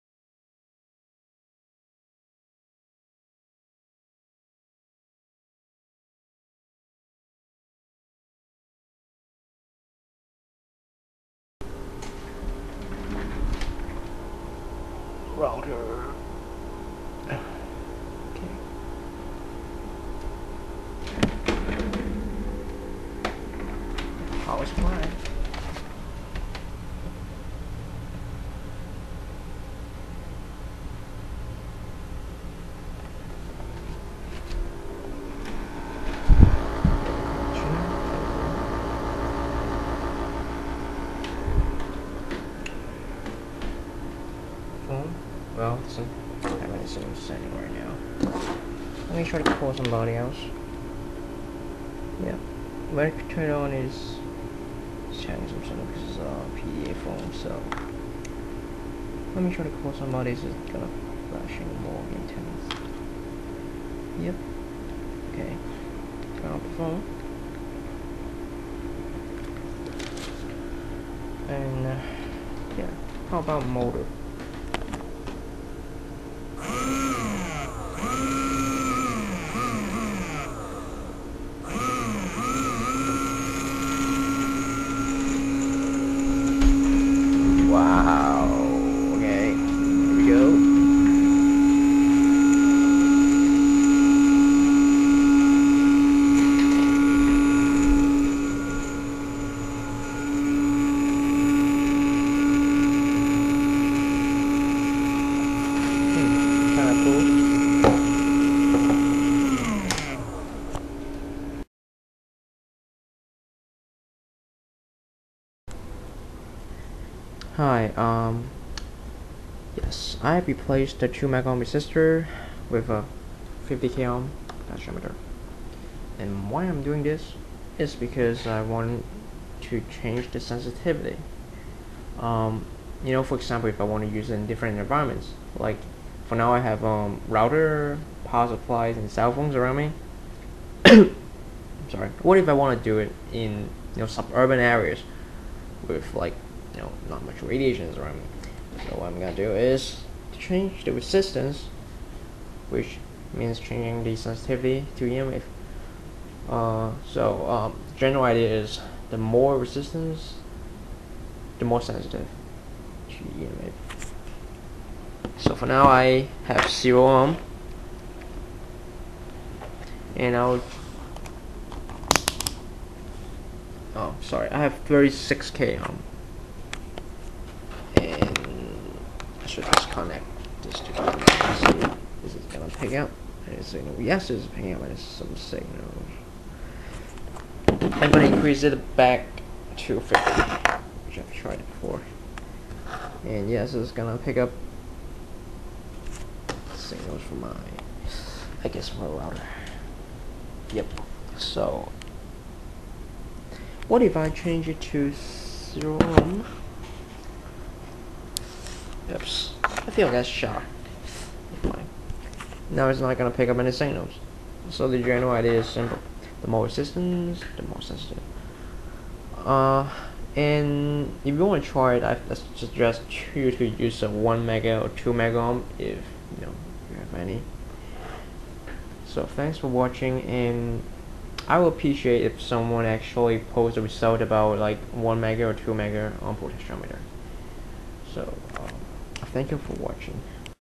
AVR 168 EMF detector
Thanks to biltoddthesciencegod, I have tried the project and I've made a little adjustment on the resistor. Now we will be able to change the EMF sensitivity so that we can use it in different enviroments.